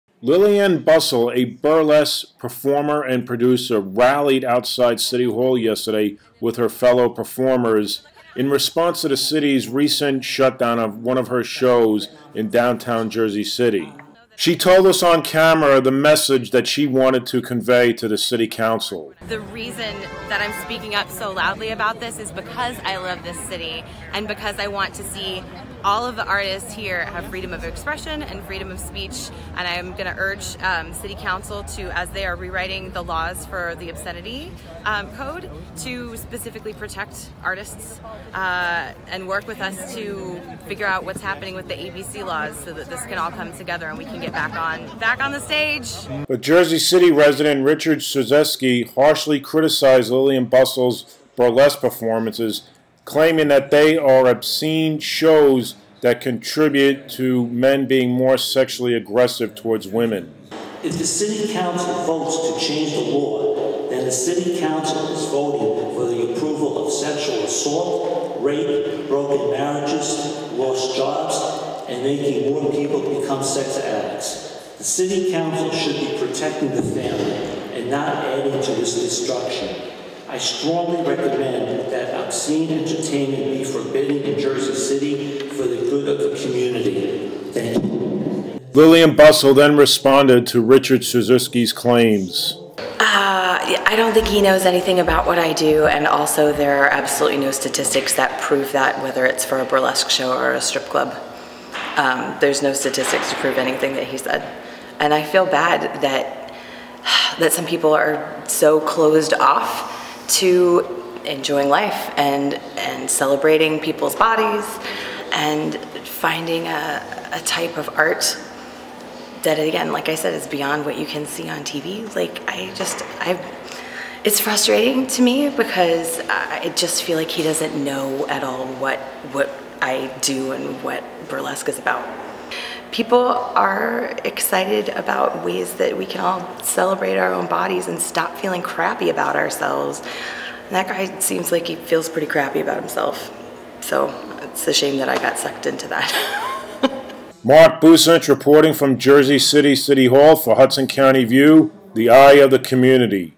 At Jersey City Council meeting, burlesque performers say the show must go on
Burlesque performer and producer Lillian Bustle, joined by a group who supports the arts, told the Jersey City City Council yesterday to reinstate her shows to preserve freedom of expression for artists.